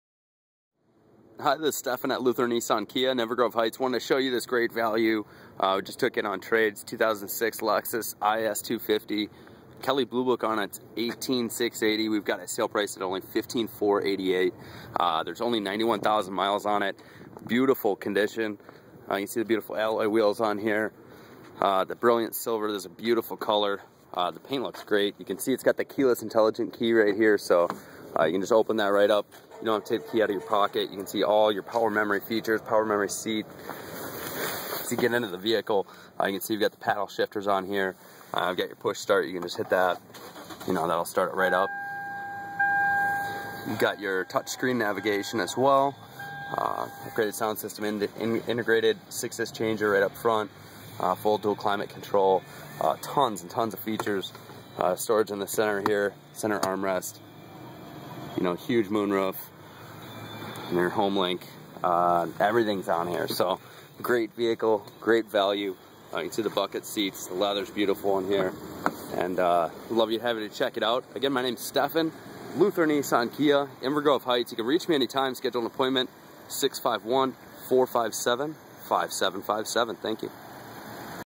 2006 Lexus IS 250 only $15,488+ttf's! Call Stephen at Luther Nissan Kia
This local trade vehicle is a 2006 with only 91,xxx miles and almost $3,000 under Kelly Blue Book.  Comes loaded with everything from Touchscreen Navigation, Air Cooled and heated Leather seating and much more.

Call Stephen Cornell at Luther Nissan Kia